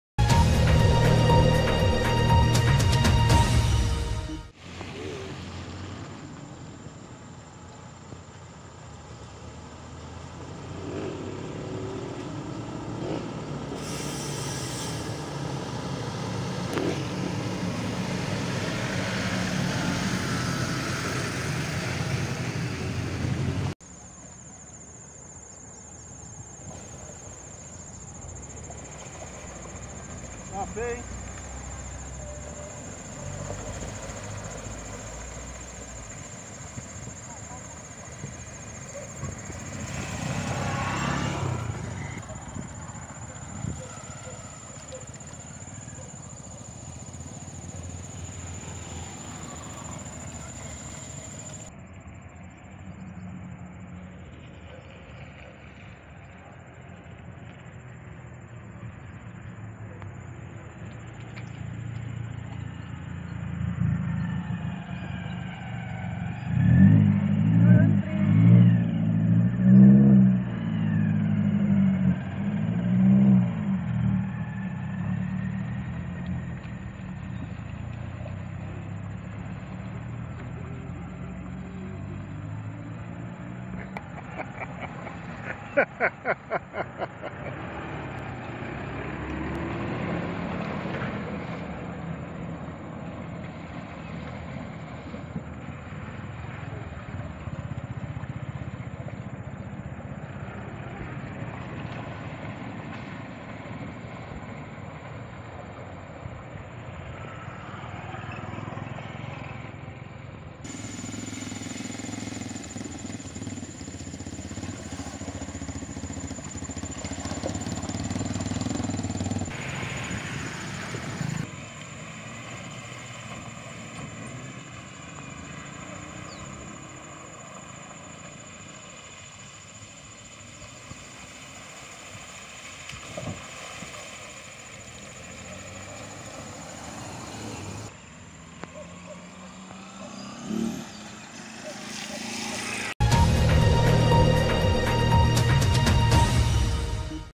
Afundamento de rua preocupa moradores da zona norte
Pavimentada com lajotas, trecho da Estrada Sonia Zenorine, situada na cidade Planejada 1, apresenta alguns pontos de afundamento que preocupam os moradores.
Eles temem que a erosão afunde, e causa uma grande cratera entre dois lagos, Tio Nicola.